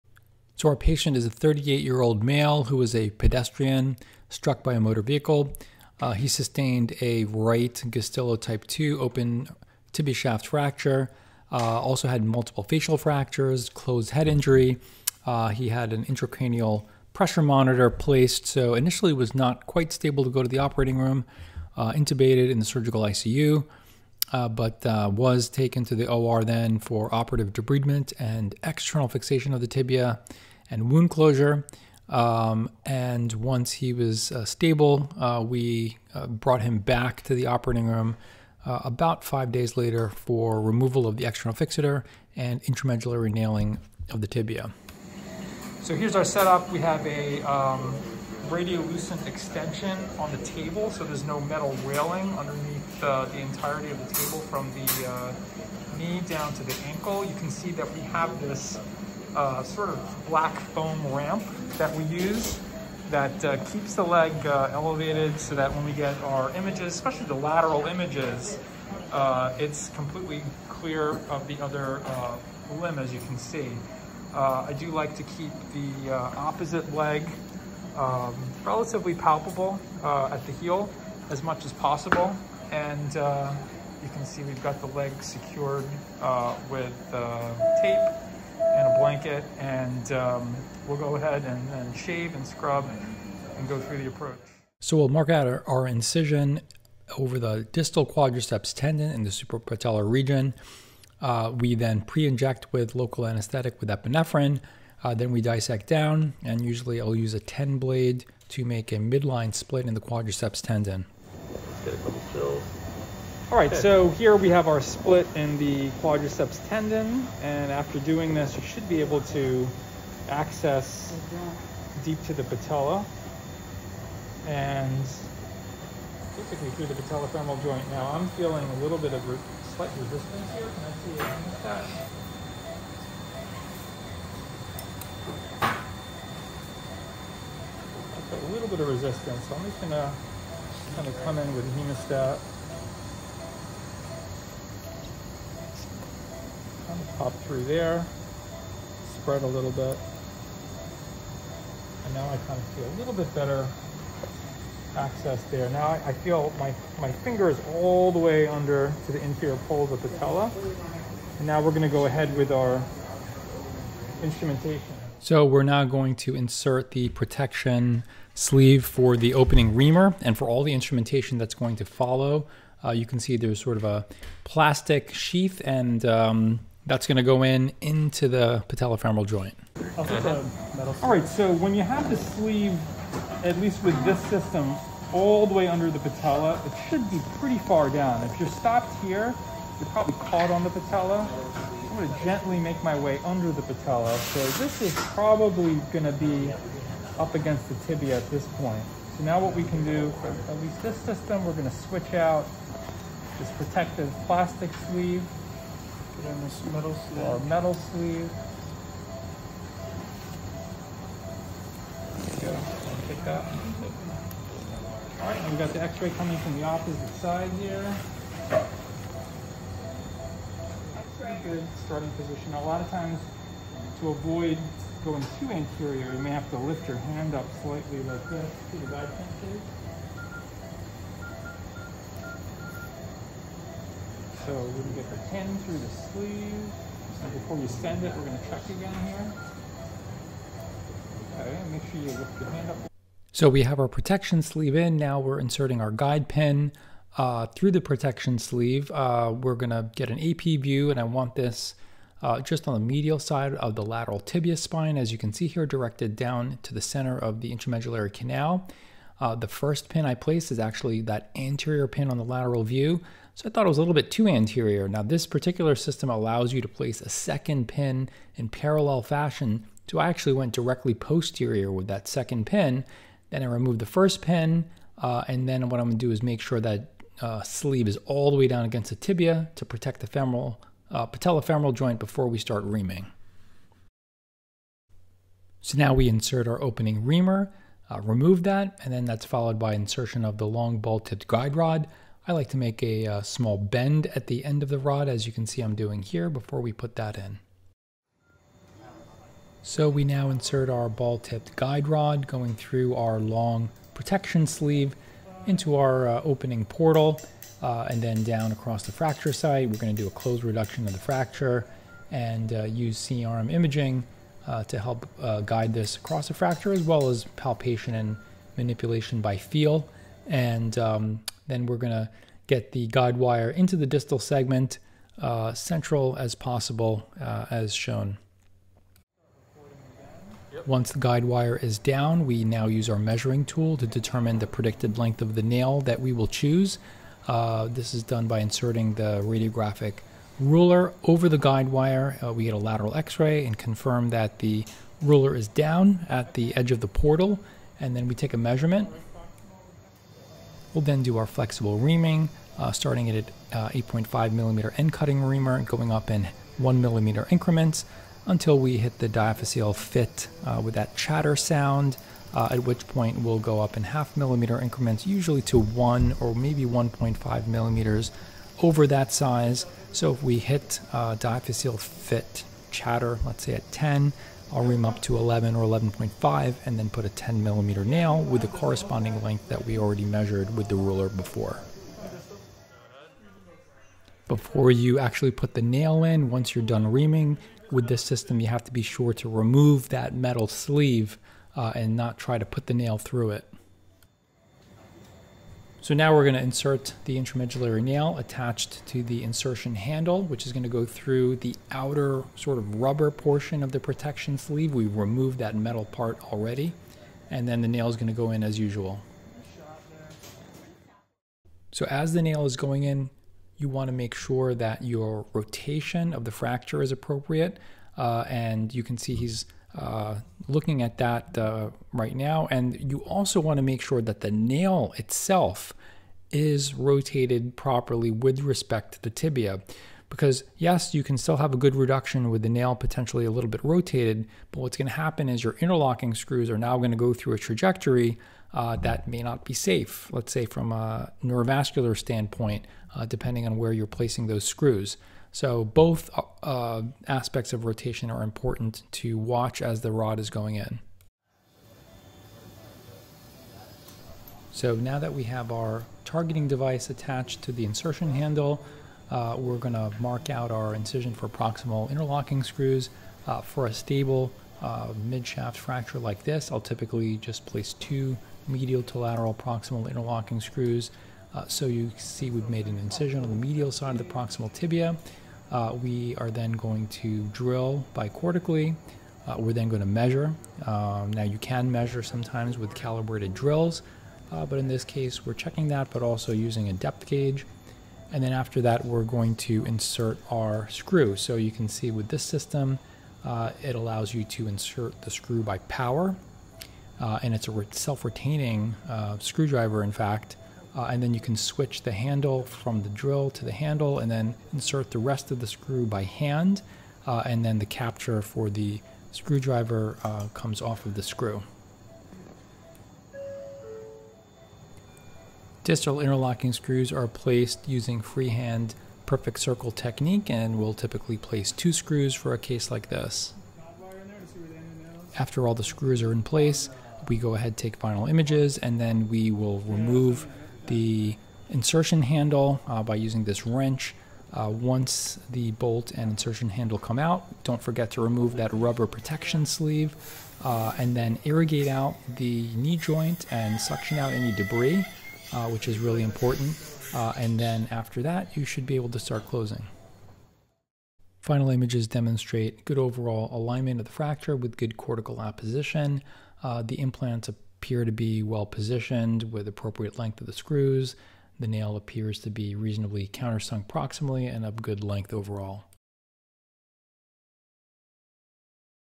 Suprapatellar tibia nailing technique
Suprapatellar nailing of a midshaft diaphyseal tibia fracture